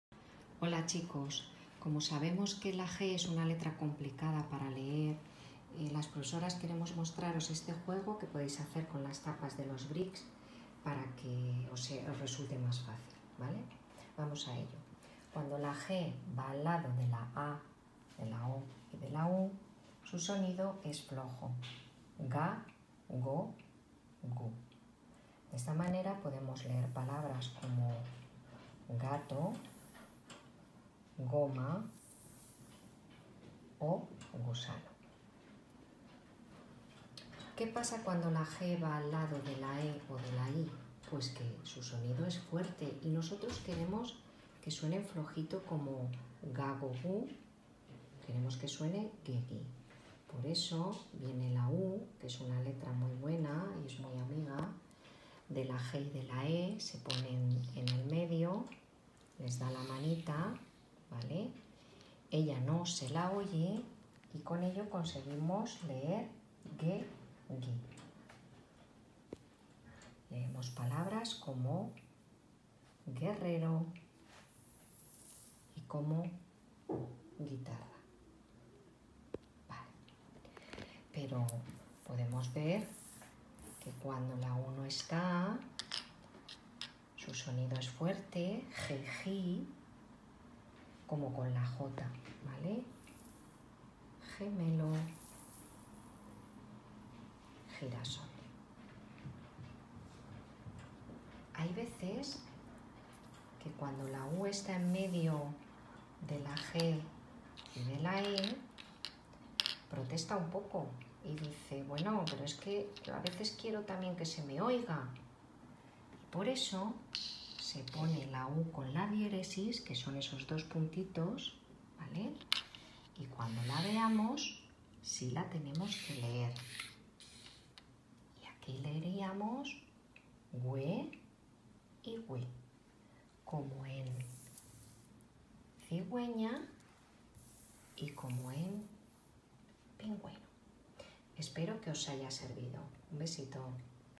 VALDEBERNARDO 3º Ed. Infantil. JUEGO DE LOS SONIDOS DE LA "G"
Aprende los distintos sonidos de la letra "g"